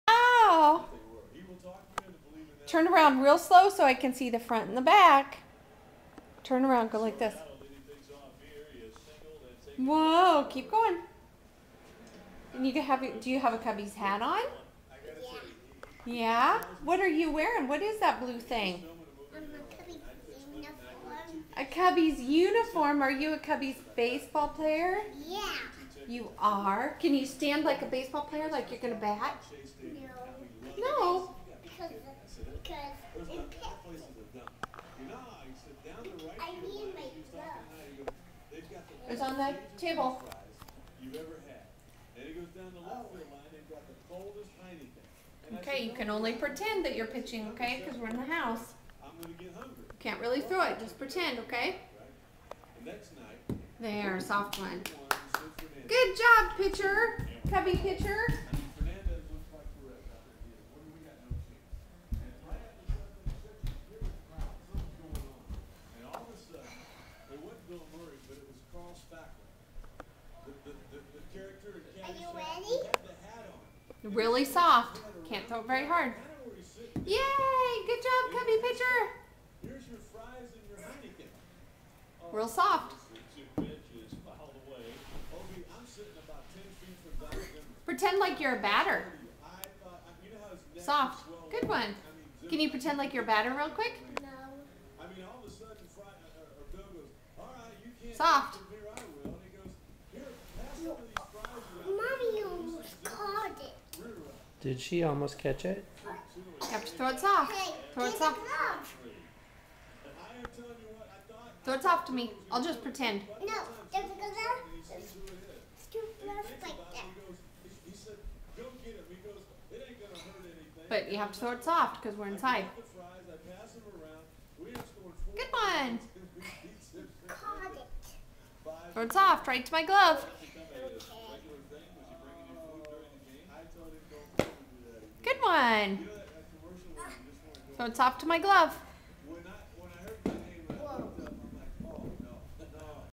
Modeling Cubs Uniform
Drake received a Cubs uniform from grandma and grandpa Miller for his third birthday. He models the uniform and shows off his pitching skills.